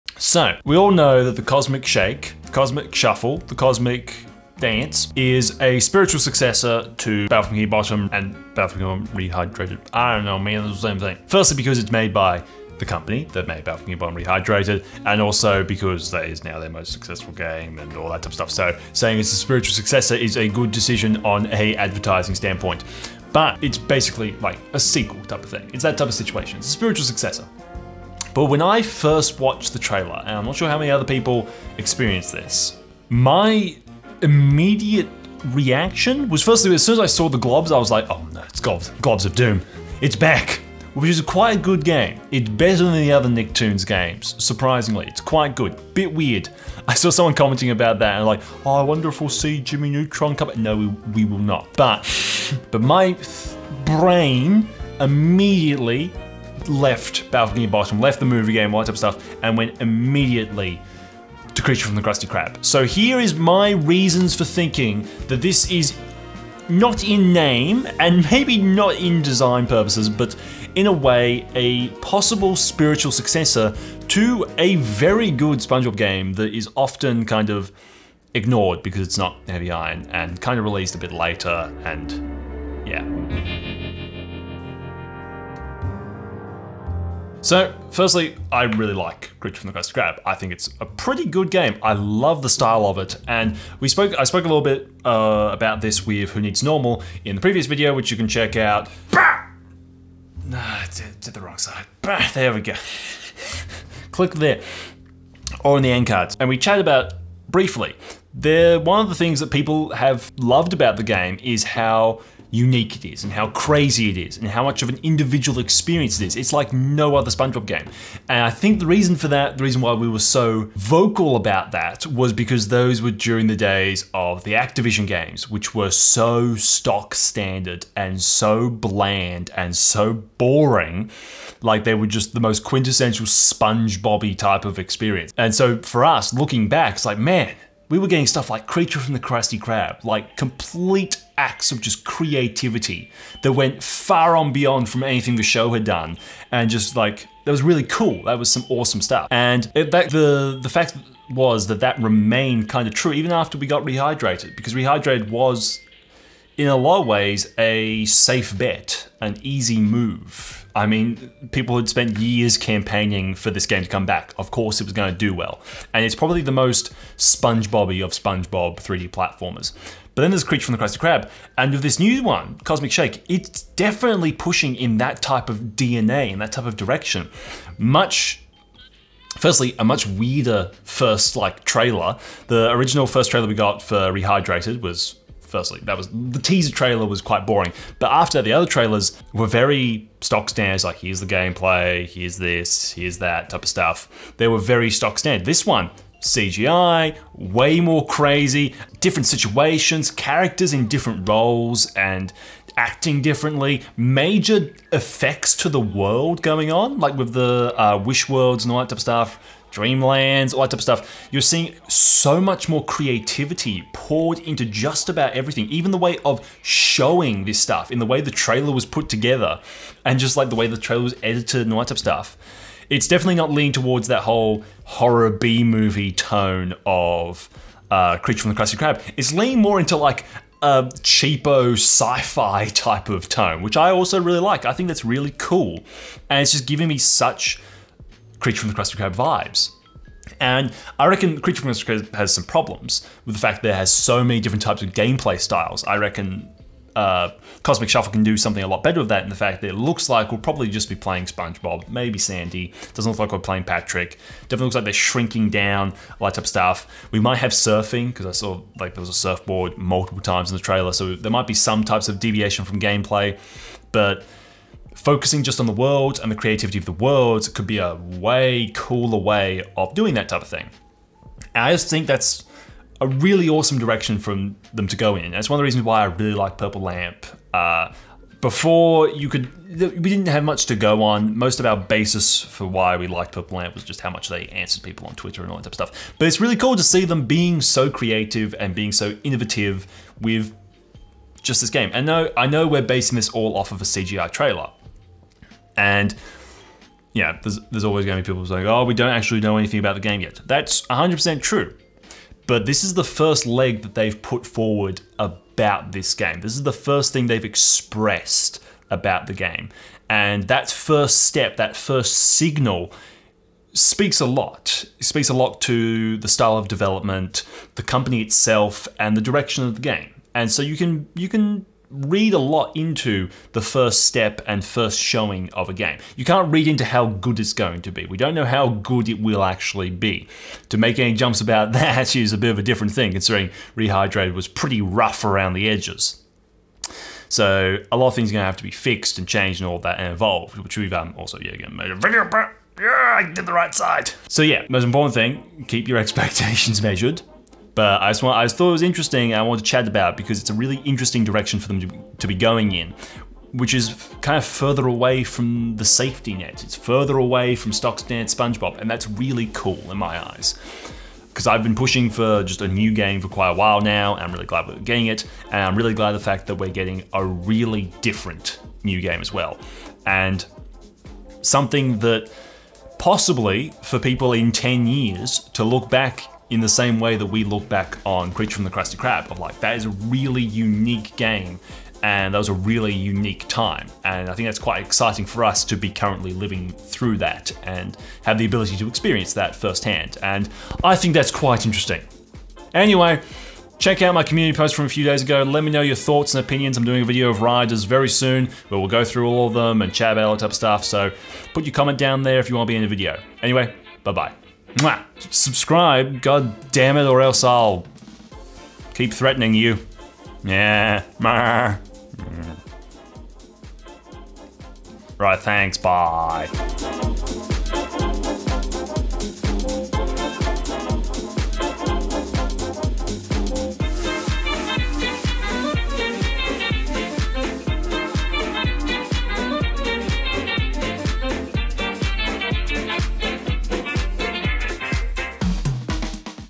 The Spiritual Succesor We Weren't Expecting! | SpongeBob: Cosmic Shake & CFTKK
So I thought I'd sit down and chat about something that's been on my mind since the release of the SpongeBob SquarePants: Cosmic Shake trailer and that's how it is already showing a fair few similarities to one of the great SpongeBob games of the past, SpongeBob SquarePants: Creature From The Krusty Krab! So let's have a little chat about it!